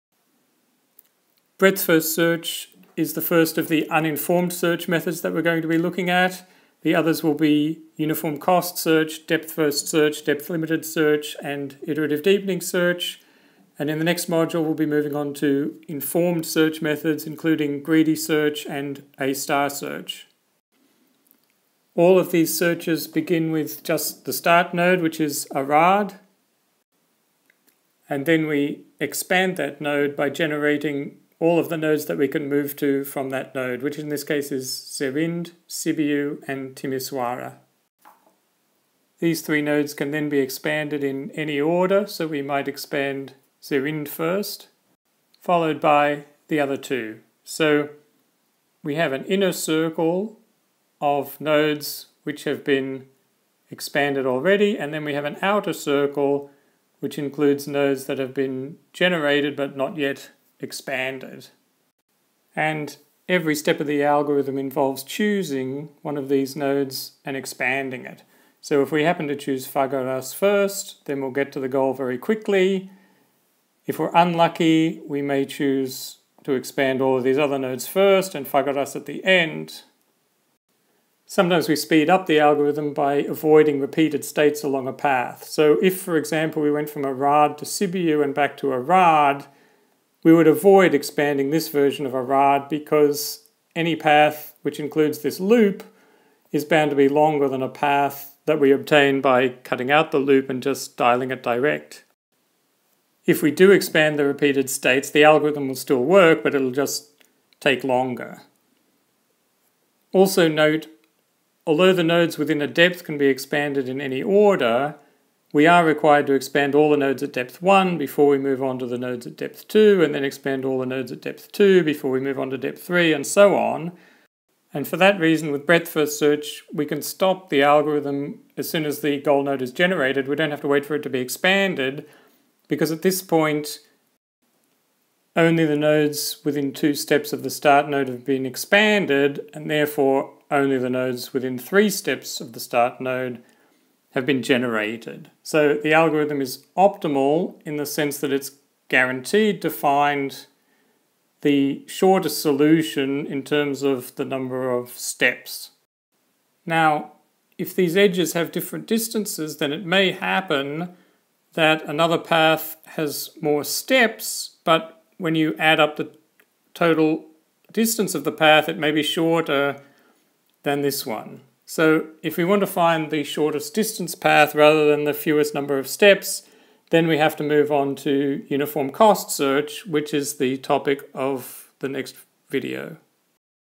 Breadth First Search - Alan Blair, UNSW
Breadth First Search, based on Romania Map from Russell & Norvig